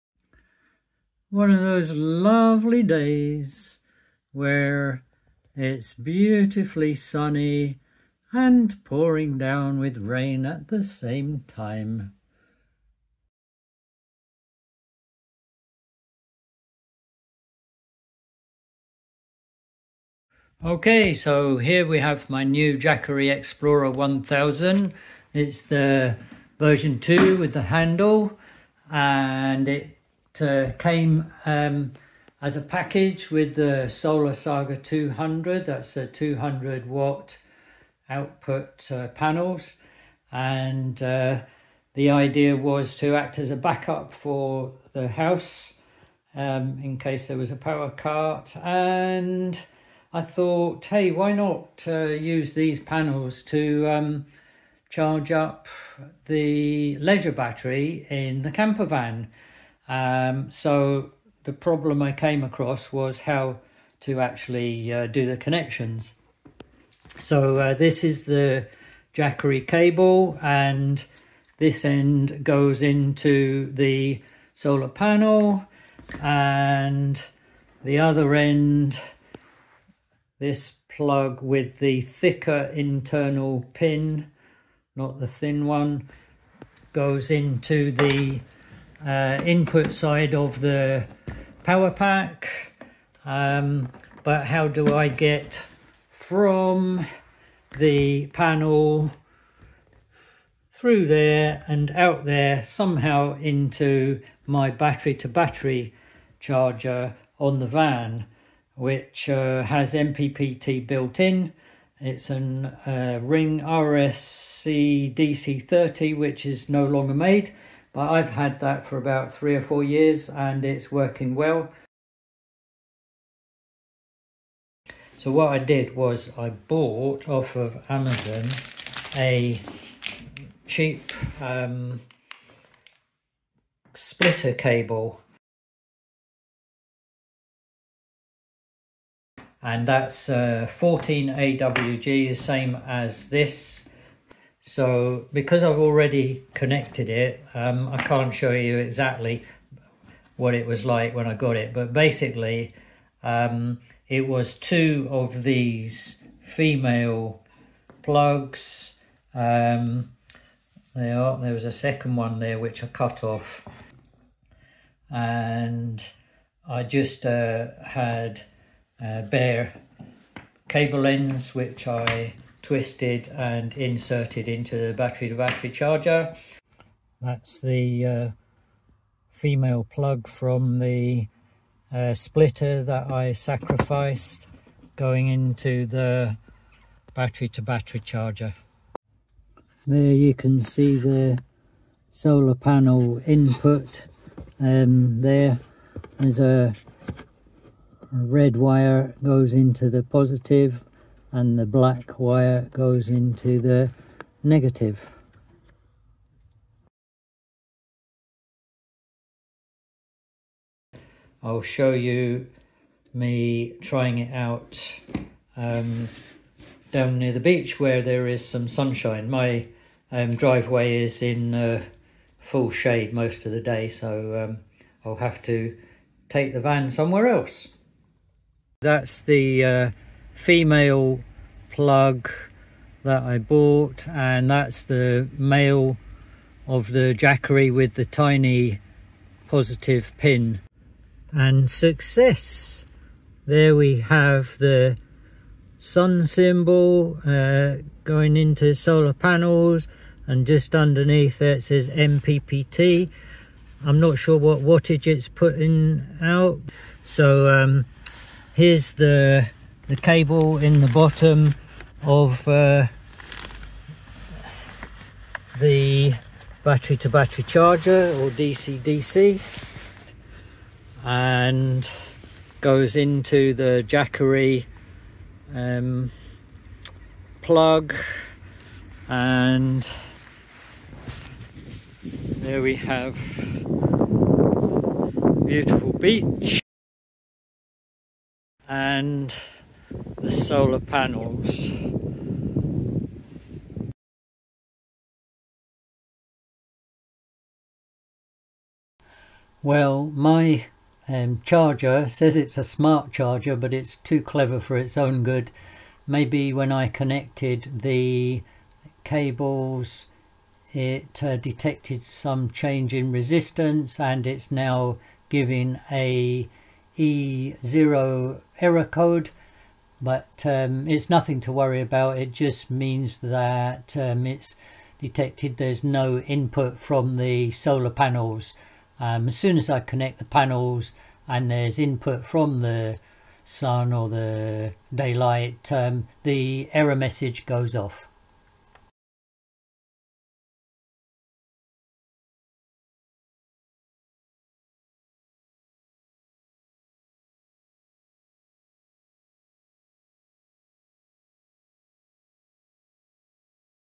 Charge van leisure battery with Jackery solar panels via MPPT controller and what went wrong!
Using a third party cable to connect van leisure battery with Jackery Solar Saga panels via Battery to Battery MPPT charge controller .
CAUTION: I am not a qualified electrician or electronics engineer so this is not advice but a 'How I did it' video.
My B2B is a 4 year old Ring Automotive RSCDC30 that is no longer made. It usefully has input connectors and MPPT on board.
I needed bare cable ends to feed in to the connectors and it did not seem that I could buy anything off the shelf with the correct 8mm female plug on the other end.
So I bought a £10 solar splitter cable off Amazon and cut off the part I needed. It is much shorter than the photo in the listing.
Now I have found that it worked, I will put an inline breaker on the positive side in this third party cable. I'm not sure it's needed but will do it anyway.
Lee